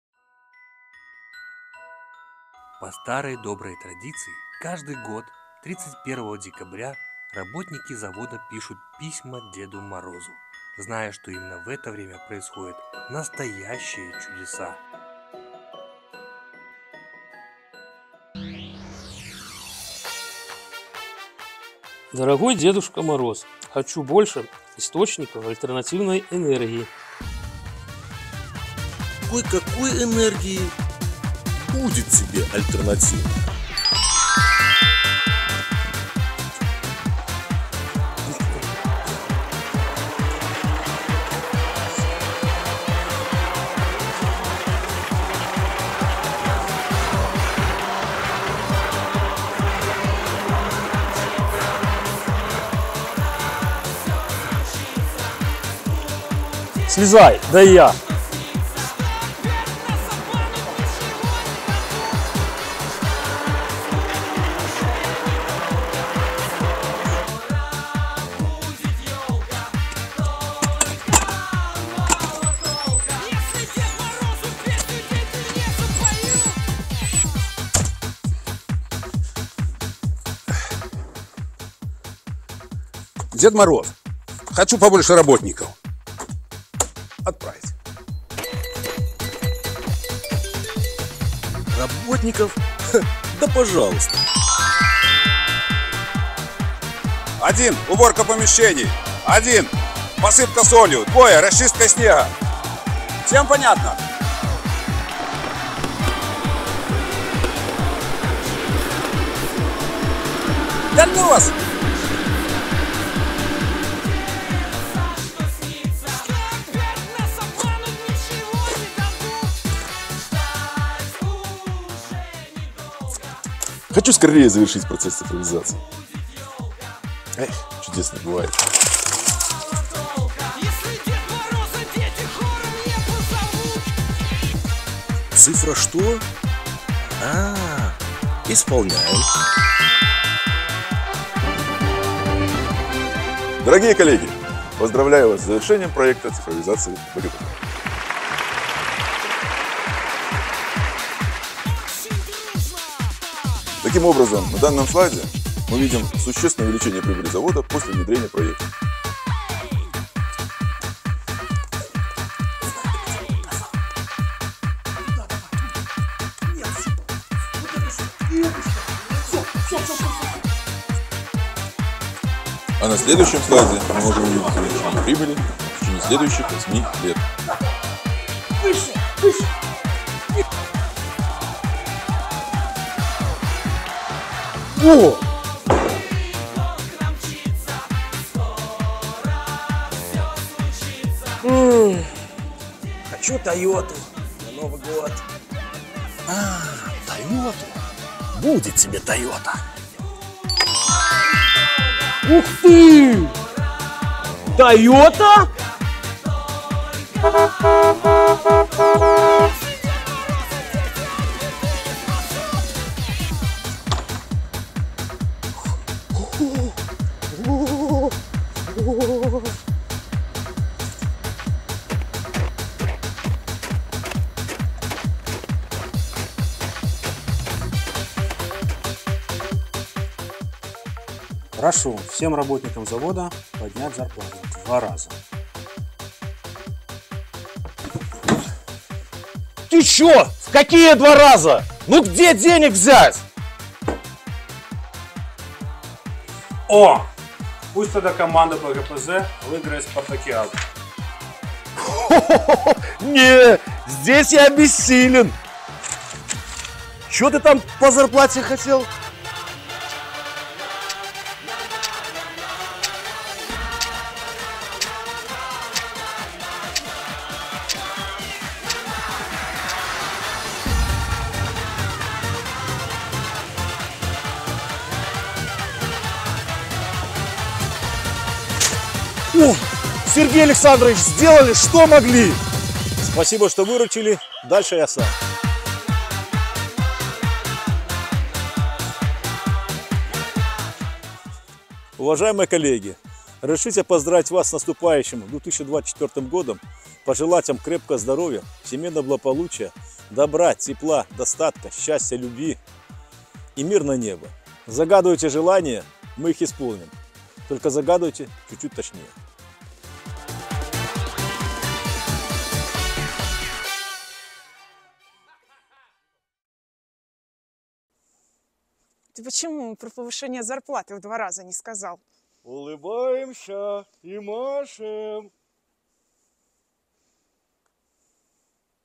Встречаем 2024-й вместе! // Белорусский газоперерабатывающий завод
Корпоративный марафон поздравлений продолжает коллектив Белорусского газоперерабатывающего завода.

🌲 В течение месяца присылайте в телеграм-канал «Инфоповоды_Belorusneft_official» или в чат-бот @Belorusneft_info_bot поздравительный ролик, снятый горизонтально.

🌲 При необходимости файл снабжается пояснительным комментарием. Поздравительный марафон продлится до конца декабря 2023 г.  Видеооткрытки украсят официальные медиаплощадки «Белоруснефти».